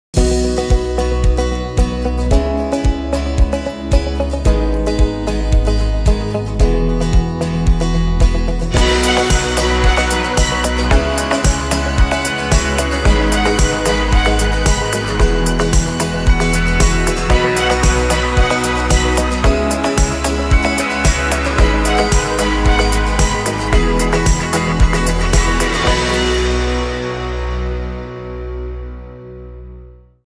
Betashares Nasdaq 100 ETF (ASX: NDQ)
Seeking exposure to global technology markets? Watch this short video to learn about how the Betashares Nasdaq 100 ETF (ASX: NDQ), offers exposure to the global technology sector with companies like Google, Facebook and Amazon - all in one single, cost-effective trade.